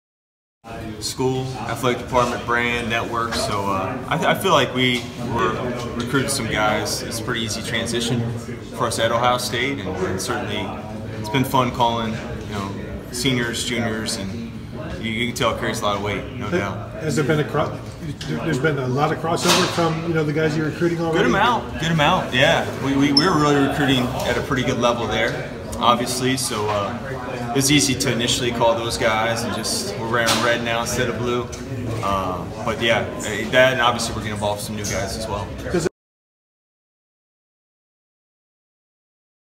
New Ohio State basketball assistant Mike Schrage talks early recruiting push
Schrage and the rest of Chris Holtmann's staff from Butler joined the new coach at Ohio State.